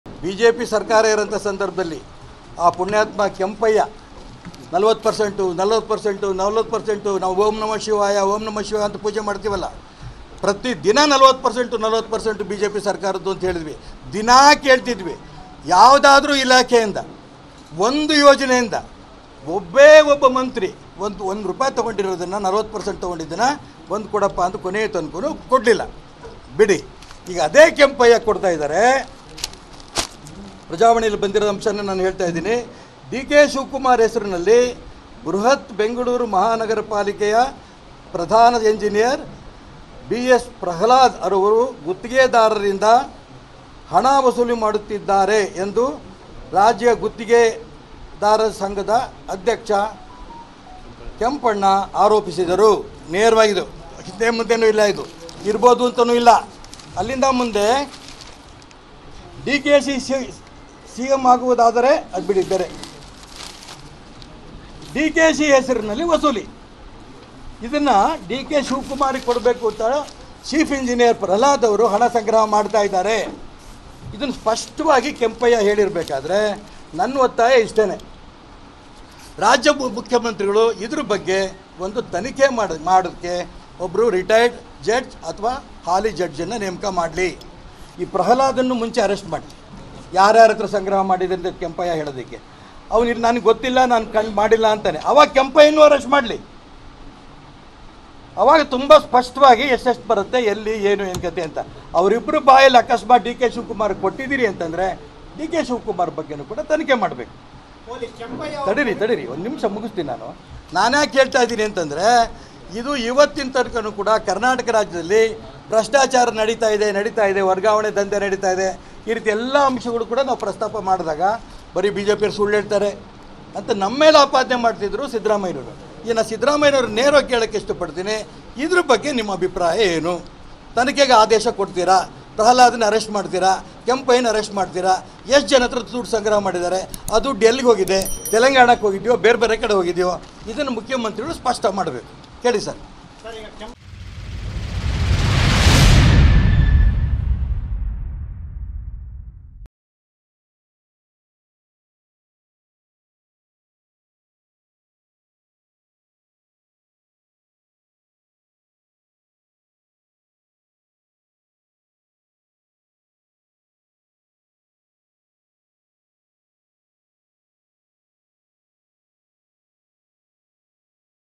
Eshwarappa Demands Probe Against DK Shivakumar | ಗುತ್ತಿಗೆದಾರರಿಂದ ಹಣ ವಸೂಲಿ ಬಗ್ಗೆ ಡಿಕೆಶಿನ ತನಿಖೆ ಮಾಡಲಿ
Eshwarappa Demands Probe Against DK Shivakumar | ಗುತ್ತಿಗೆದಾರರಿಂದ ಹಣ ವಸೂಲಿ ಬಗ್ಗೆ ಡಿಕೆಶಿನ ತನಿಖೆ ಮಾಡಲಿ: ಕೆ.ಎಸ್​. ಈಶ್ವರಪ್ಪ...! BELLARY